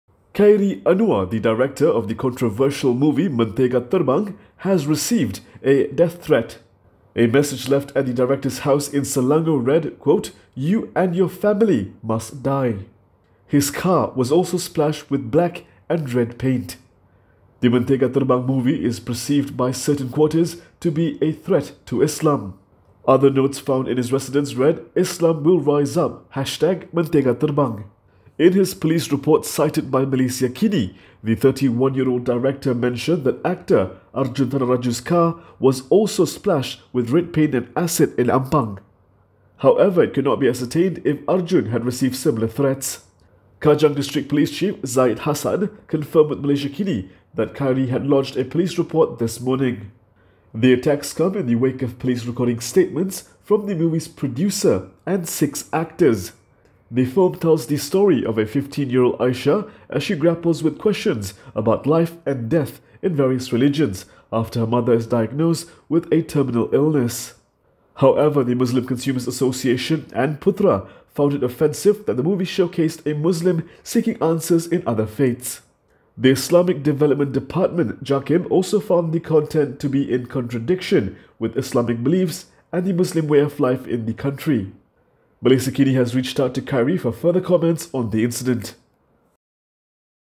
Mentega Terbang director receives death threat, car splashed with paint
“You and your family must die”, read one of the messages left behind at director Khairi Anwar’s house in Selangor. His car was also splashed with black and red paint.

Khairi is the director of ‘Mentega Terbang’, a controversial movie perceived by certain quarters to be a threat to Islam.